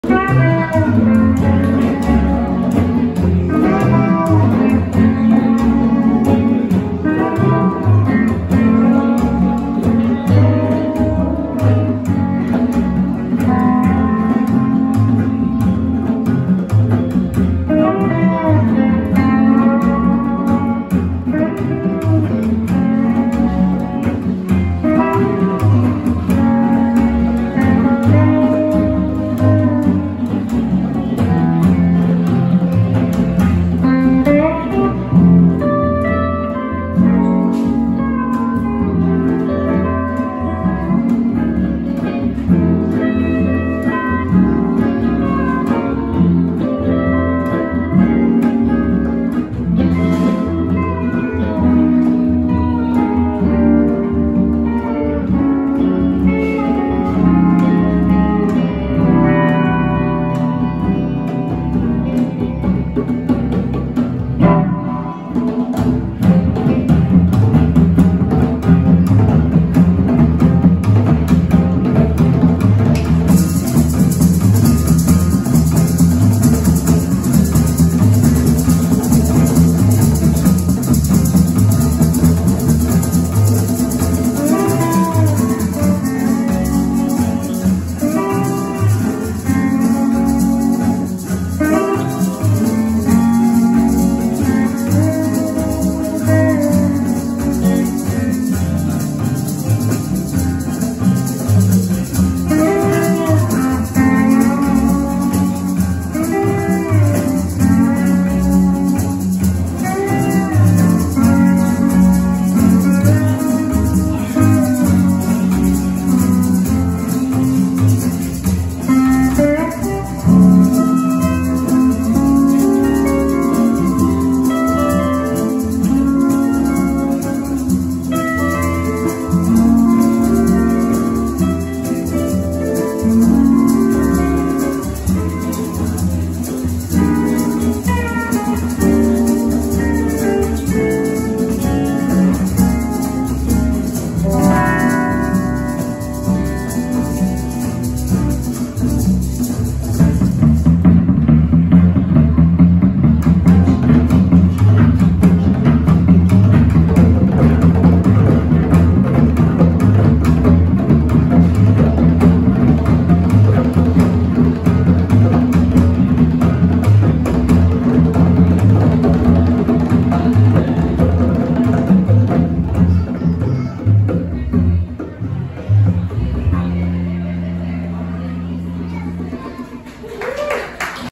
ExoTiki at at the Carlsbad Windmill Featuring Thomas Yearsley of the Paladins
ExoTiki playing at a SparCustom Productions Sunday Car show. This happened April 16, 2023, at the Windmill in Carlsbad, Ca. This show happens every 3rd Sunday of the month. Bring out your classic  car and catch some great live music. For more event  info, sparcustoms.com
The band members are:
Thomas Yearsley on the Slide Steel Guitar.
Charles Burton on Backing Guitar
Justice Guevara on Stand Up Bass
Kevin Williams on Backing Guitar
Boyce Hartsell on Conga Drums
Billy Watson on Bongo Drums.